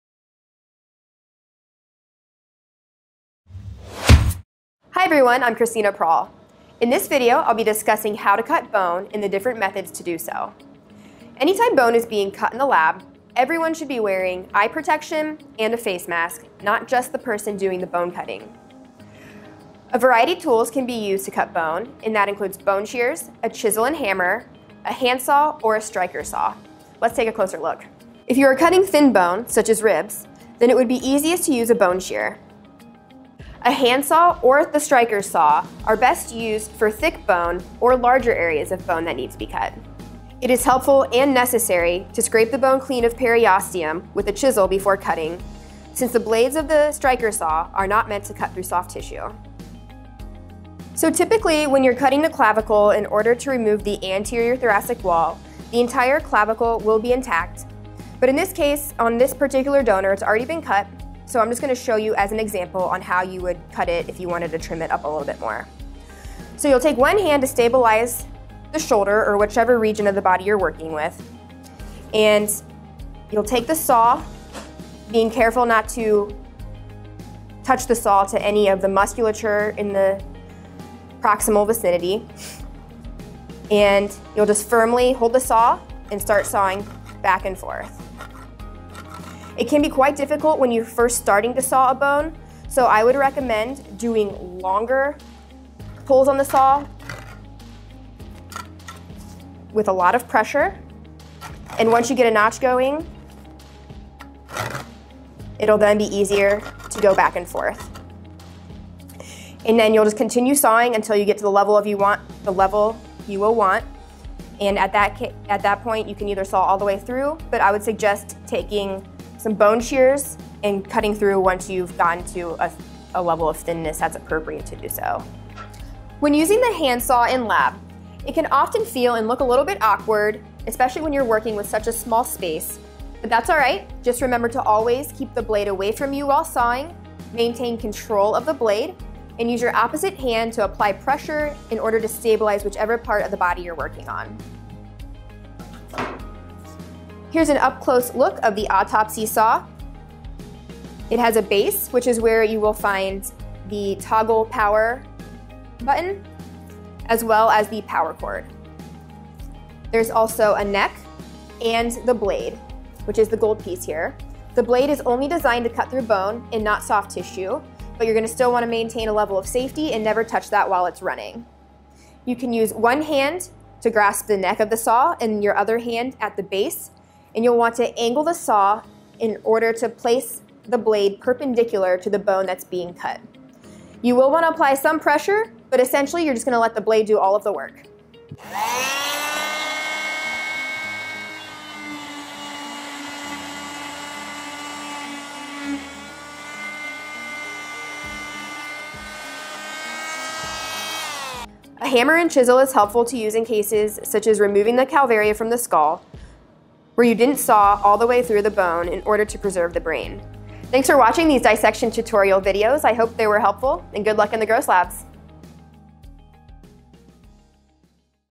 Dissection Basics | Cutting Bone
BlueLink lab coordinator Cristina Prall teaches students how to cut bone using various tools in dissection.